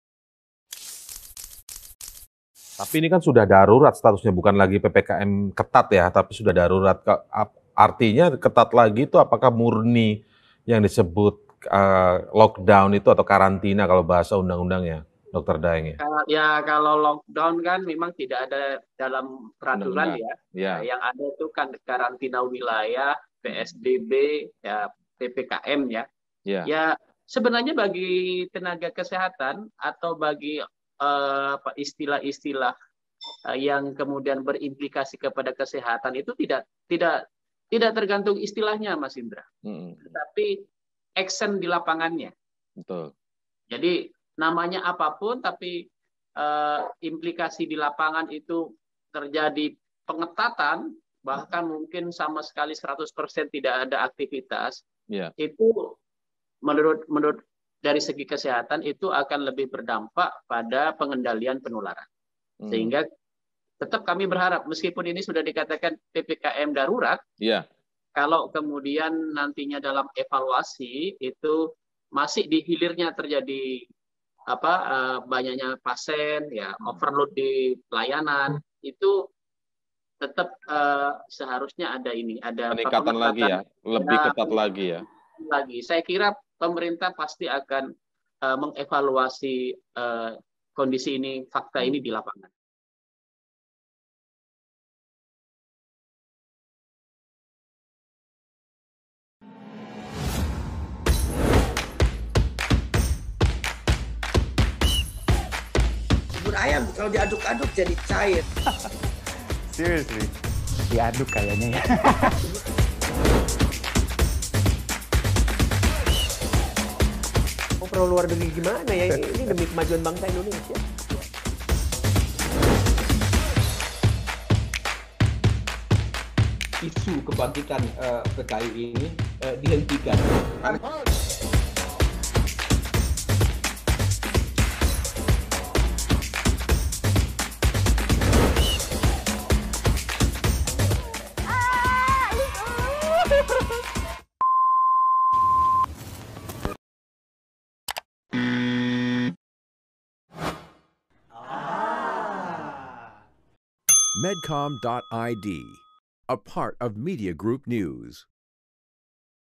PPKM Darurat Sama dengan Lockdown?
Pemerintah menerapkan PPKM Darurat yang aturannya lebih ketat dari sebelumnya. Apakah PPKM Darurat ini sama dengan lockdown? Ketua Umum Ikatan Dokter Indonesia Daeng Muhammad Faqih menjawab istilah lockdown tidak ada dalam aturan yang berlaku di Indonesia.

Daeng pun menyatakan sebenarnya istilah-istilah tersebut tidak terlalu penting. Sebab yang penting adalah implementasi di lapangan yang bisa berdampak pada pencegahan penularan COVID-19.